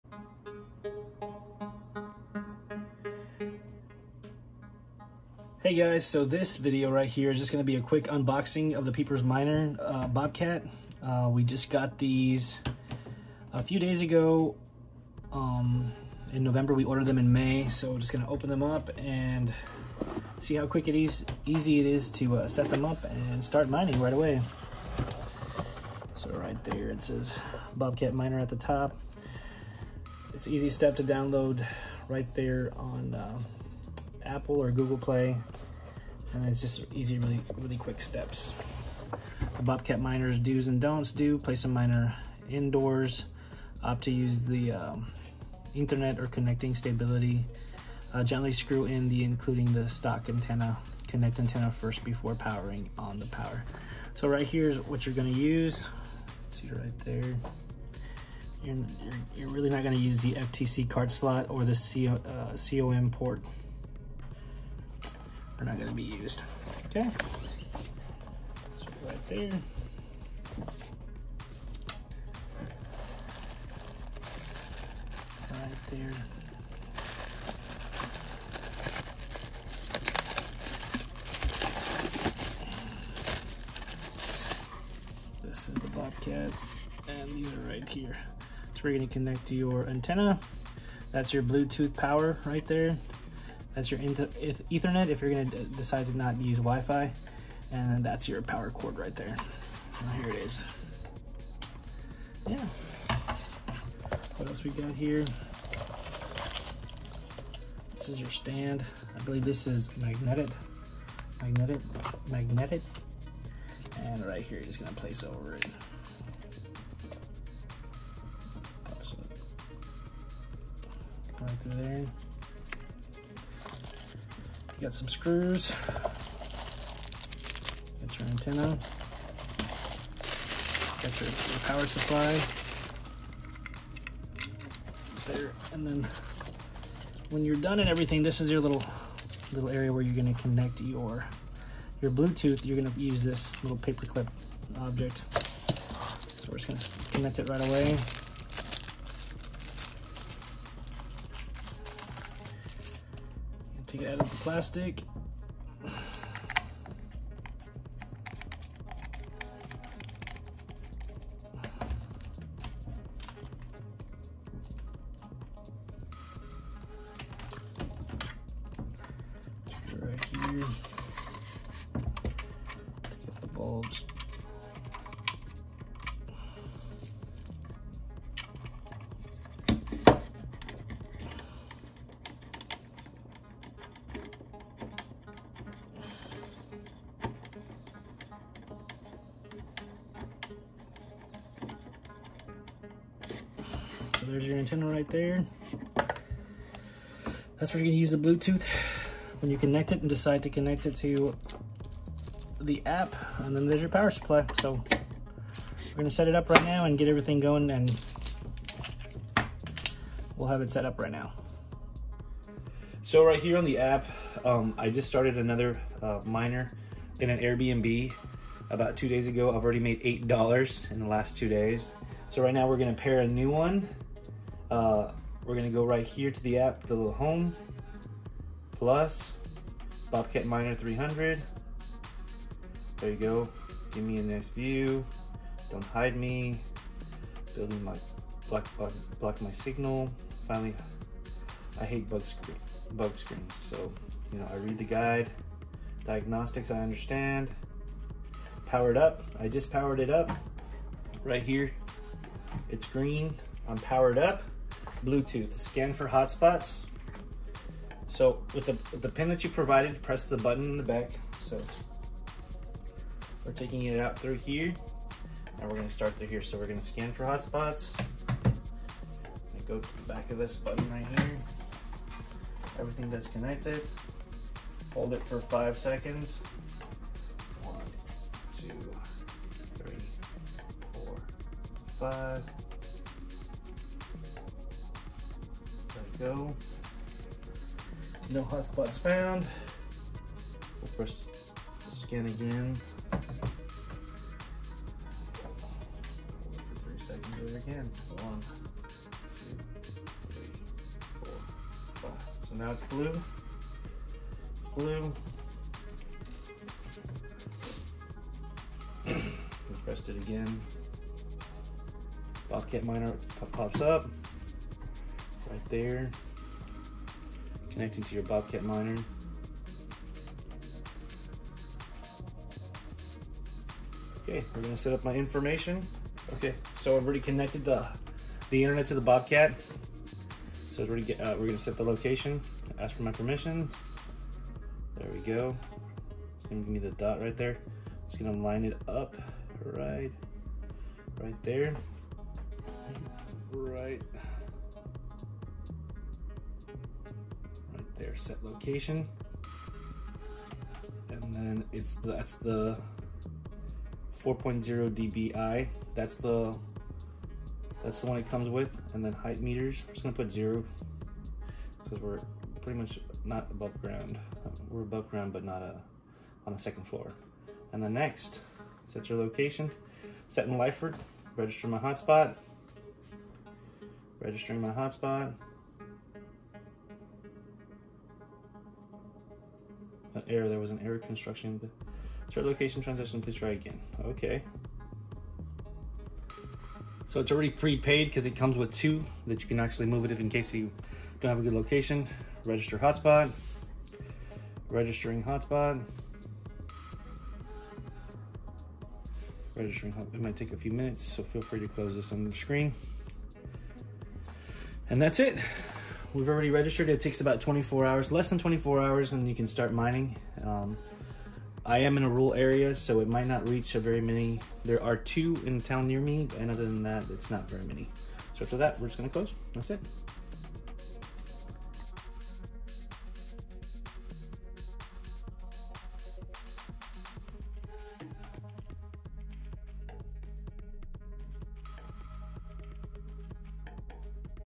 BOBCAT MINER 300 THE PEOPLE'S MINER UNBOXING AND SETUP THE HELIUM NETWORK.
Bobcat miner 300 the people miner unboxing and setup of the bobcat. Took about almost 7 months to come in. Want a new stream of passive income.